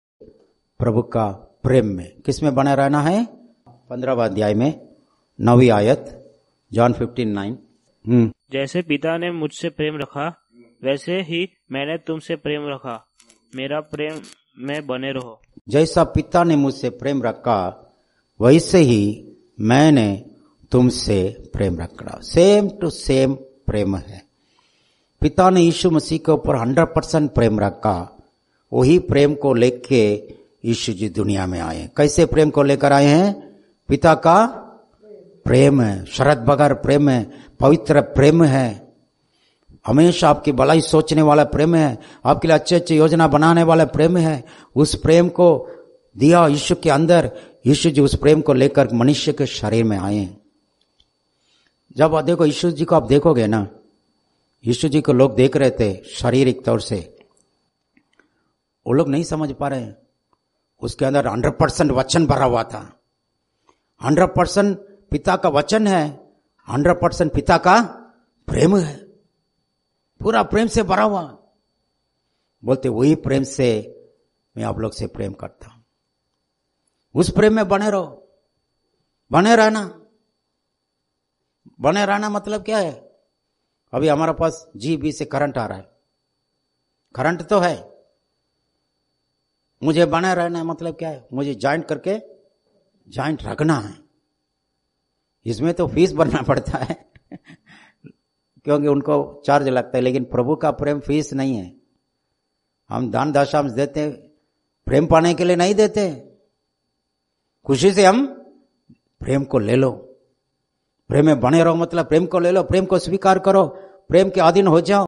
Short message on John 15;9
A small portion of sunday meeting message

Bible verse
9 जैसा पिता ने मुझ से प्रेम रखा, वैसा ही मैं ने तुम से प्रेम रखा; मेरे प्रेम में बने रहो।
यूहन्ना 15:9